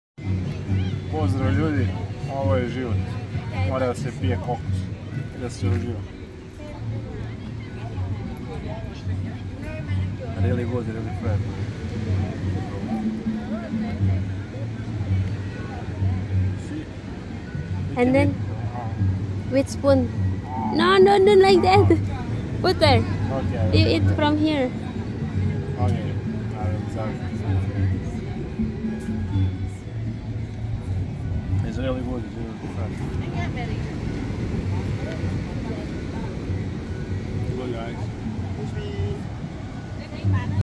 Ljudi ovo je zivot! Mora da se pije kokos! 😁🥥 Kokosovo mleko jaca potenciju! Probudi Tigra u sebi! 🐯
Ljudi ovo je zivot! Mora da se pije kokos! 😁🥥 Kokosovo mleko jaca potenciju! Probudi Tigra u sebi! 🐯 BALI 2024 COCONUT 🥥 INDONESIA 🇮🇩